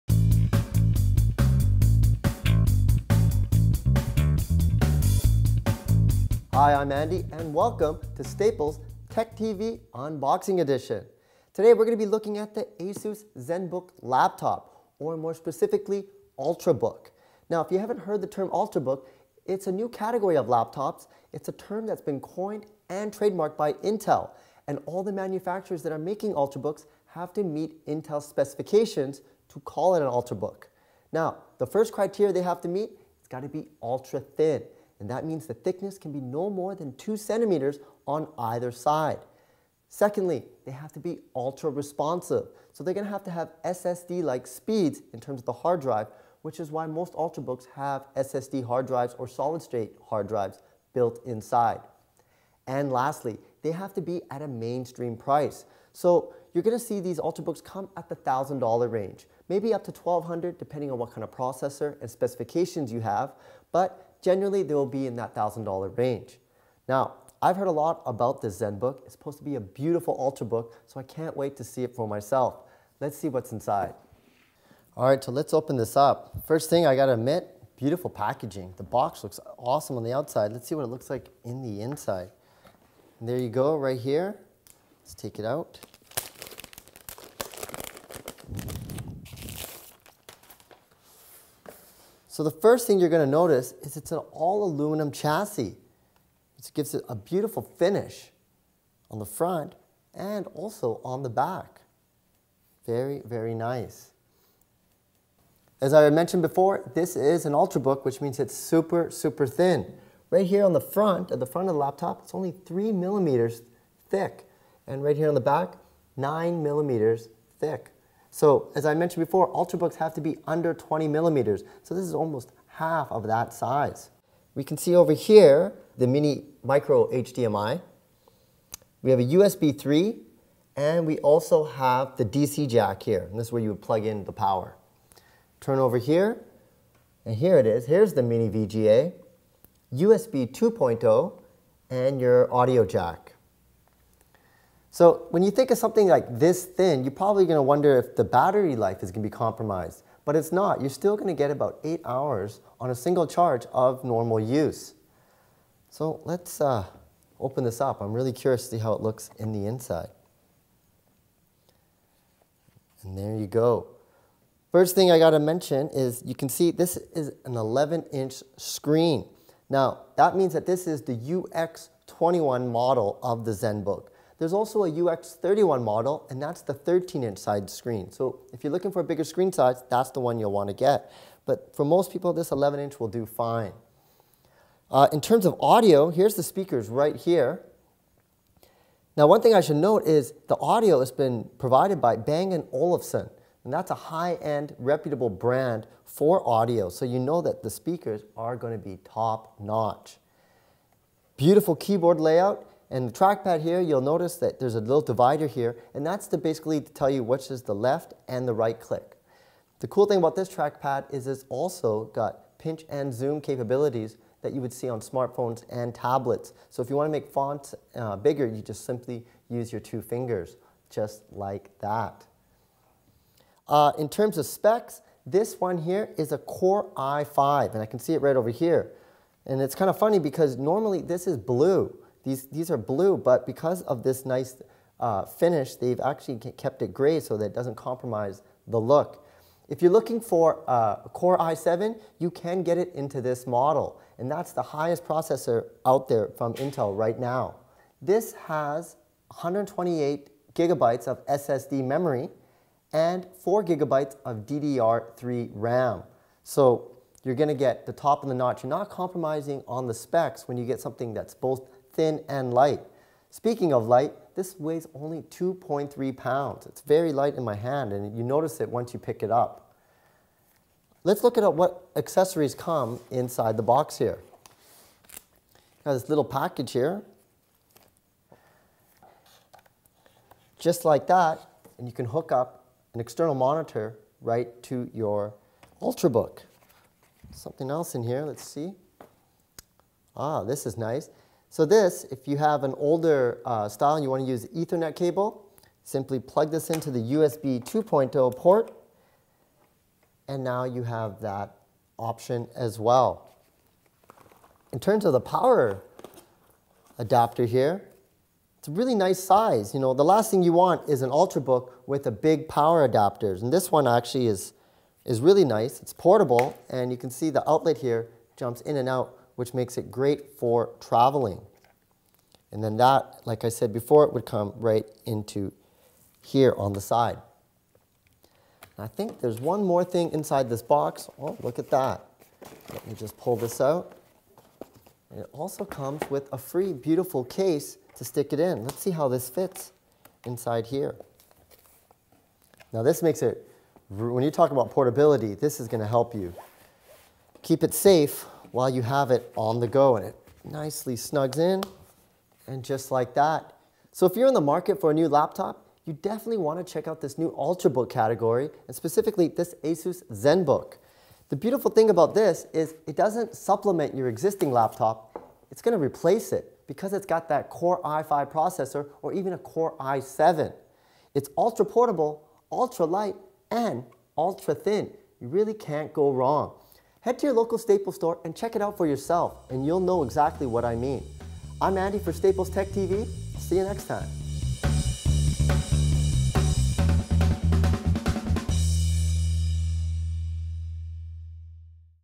Unboxing the ASUS Zenbook
for your chance to WIN great prizes each week!

Andy unboxes the ASUS Zenbook UX21 Ultrabook and explains how laptops and the new Ultrabook category are different.